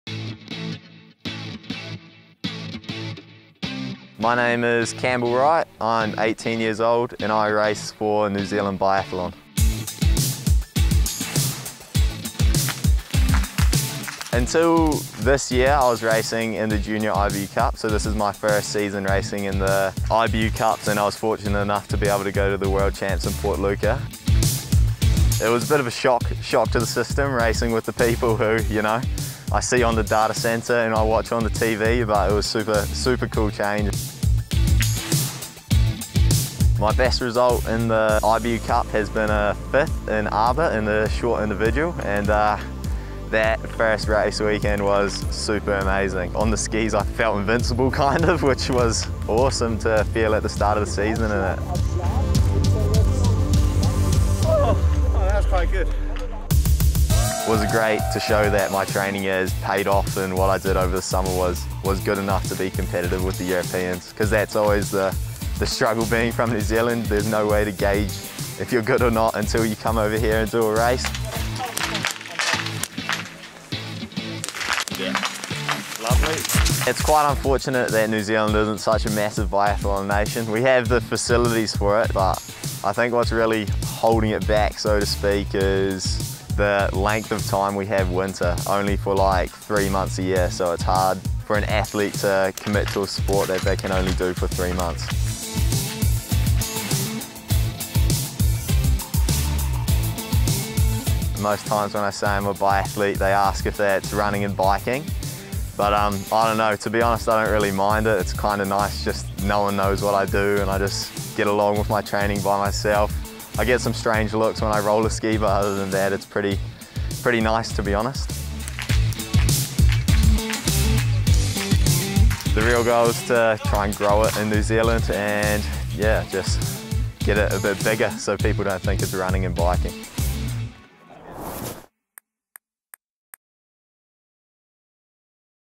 Campbell Wright - The Newcomers
Making his mark in the 2020/2021 IBU Cup season, young Campbell Wright put New Zealand on the biathlon map. The young Kiwi is now more motivated than ever for what's coming next, and hopes to change the image of the sport in his home country.